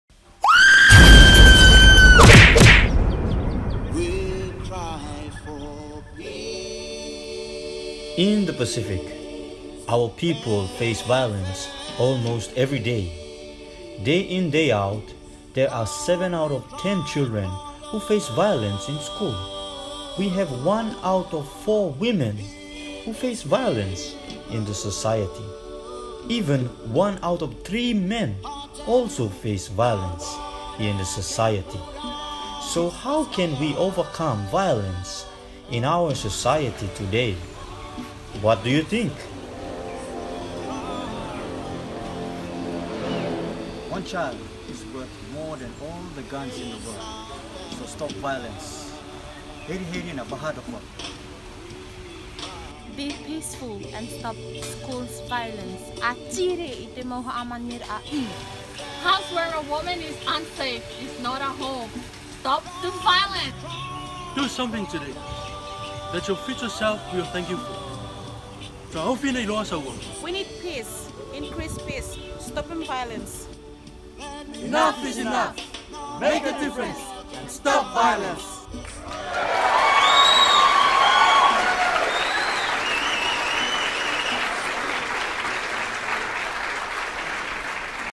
Enough is Enough - Stop Domestic Violence
Between 16th and 20th July, 2018, twenty five young men and women from eleven Pacific island countries were part of the Council for World Mission’s Pacific Regional Youth Initiative, in Nadi, Fiji, for a weeklong workshop building their capacity to use social media as part of mission in the 21st century. The workshop resulted in the production of social media elements to highlight issues affecting young Pacific islanders.
Over the week, activists and advocates for climate and gender justice shared their experiences and advice on digital storytelling and social media as a tool of social change.
Participants were introduced to the biblical contexts of social media and online communities through special Bible Studies prepared and led by Rev. Dr. Cliff Bird, Mission Secretary for CWM – Pacific.
The workshop was facilitated by Rev. James Bhagwan, past Secretary for Communication and Overseas Mission for the Methodist Church in Fiji.